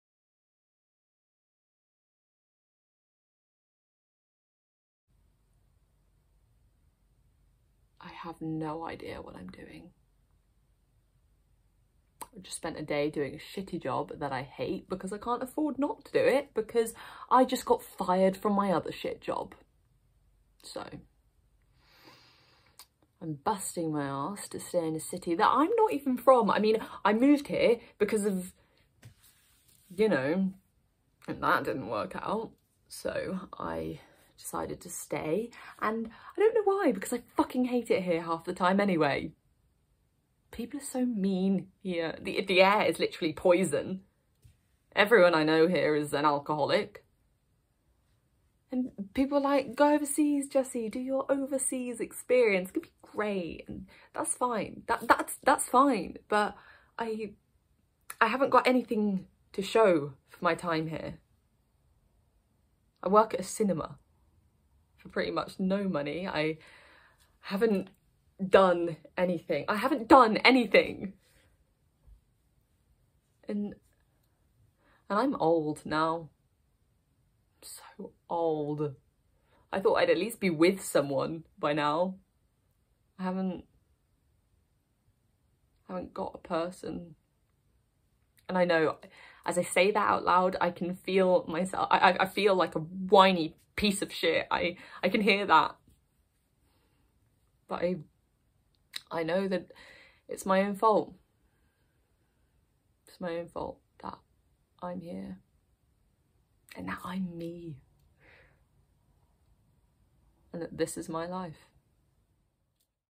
NADIA JACKSON - Starstruck monologue
Starstruck
By Rose Matafeo
Playing Jessie

2021 ALRA graduate
Currently seeking representation